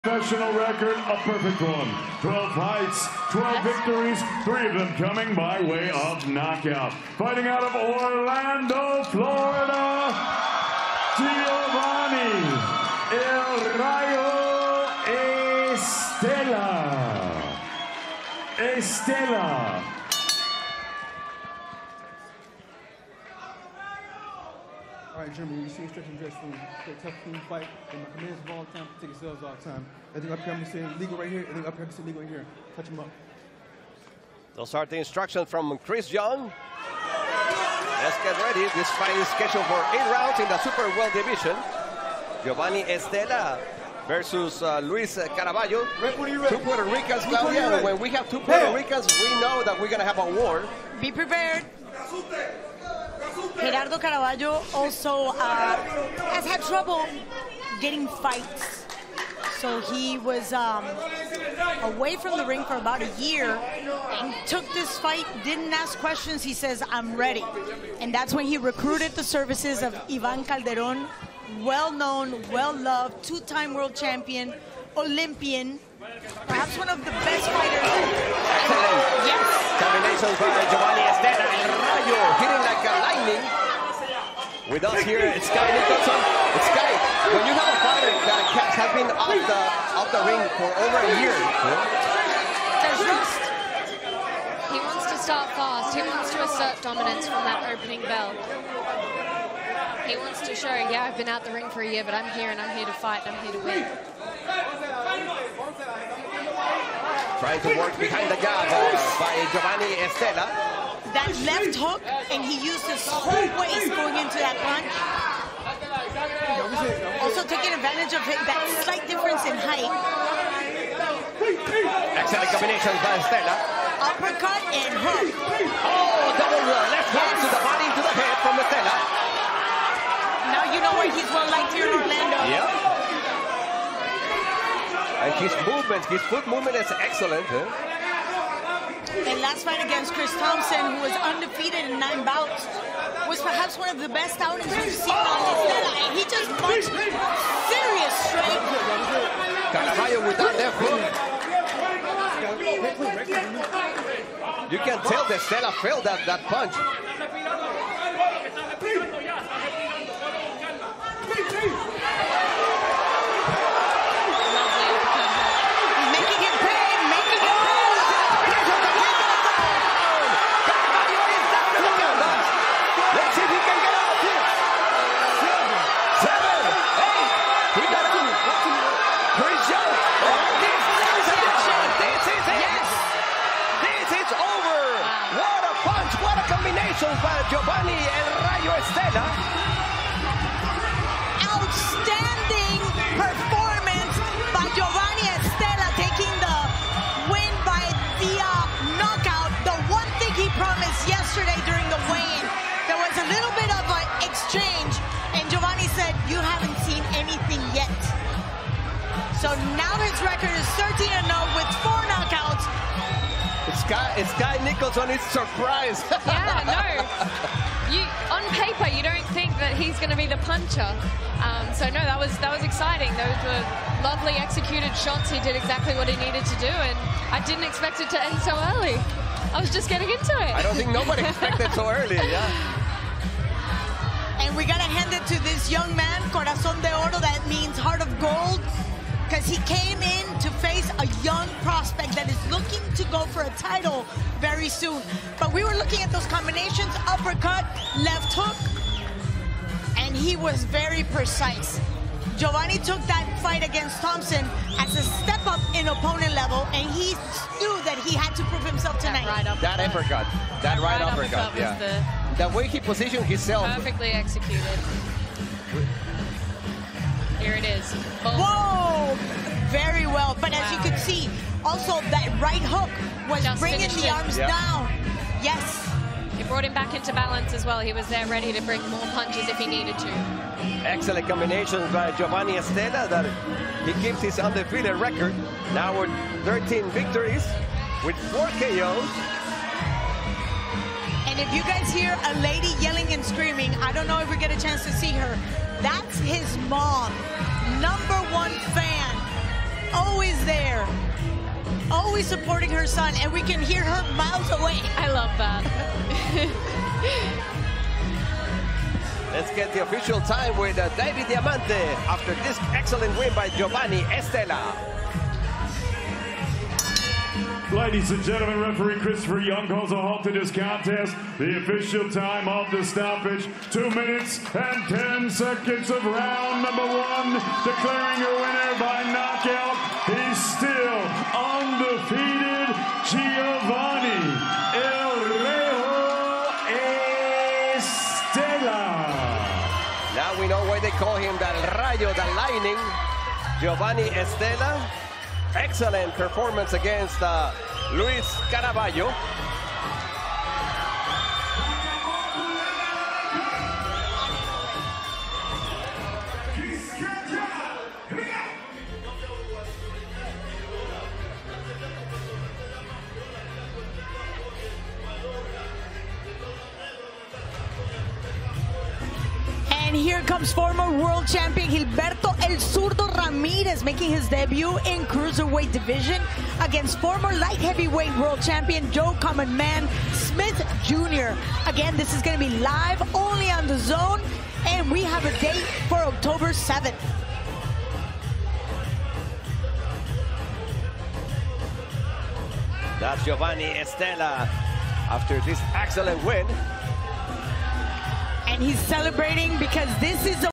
Jeovanny Estela vs Gerardo Caraballo (Hitchins vs Zepeda Undercard)
El Rayo Jeovanny Estela moves 13-0 in Orlando, beating Gerardo Caraballo in the very first round on the Richardson Hitchins vs Jose Zepeda card.